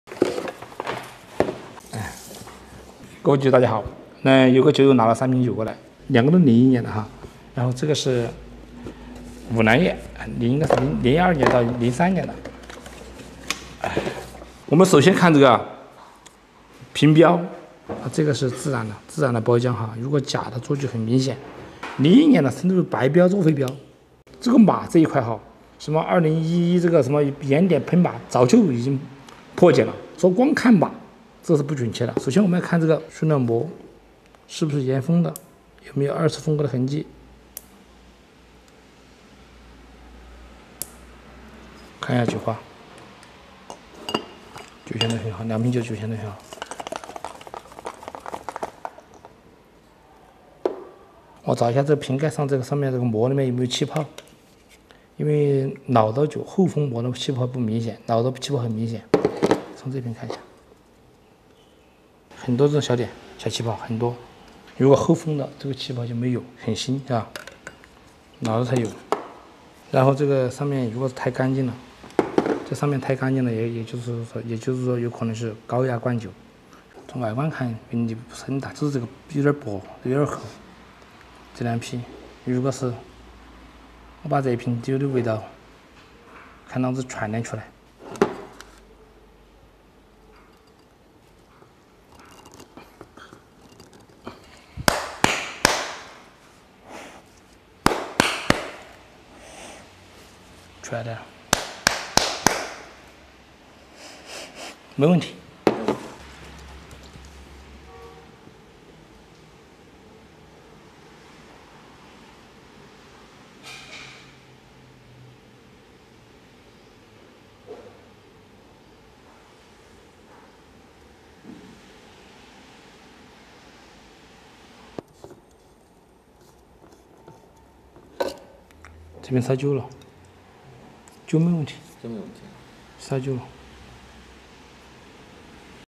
酒友鉴定2瓶白皮茅台，根哥讲解帽子封膜防伪细节，又学到一招
大家好，我是名酒根哥，带你赏遍天下好酒。